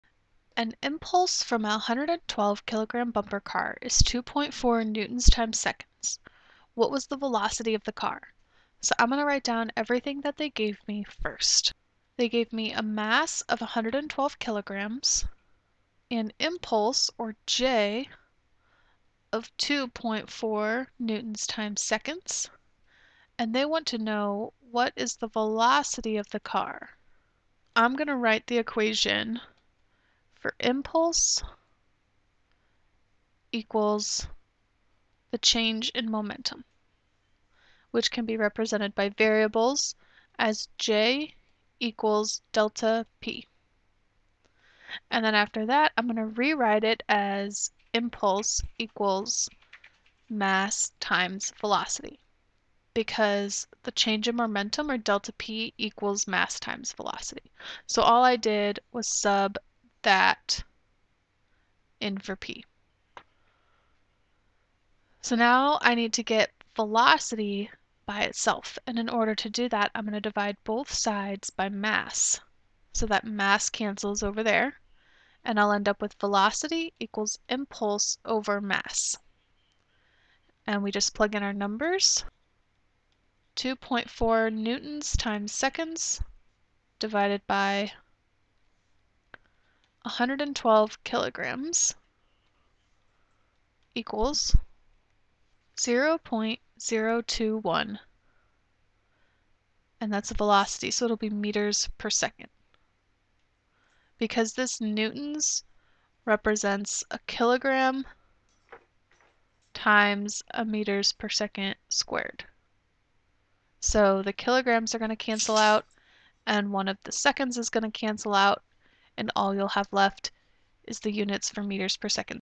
Impulse-Momentum
Shows how to find the velocity of an object when given impulse and mass. Refers to #5 on the momentum worksheet.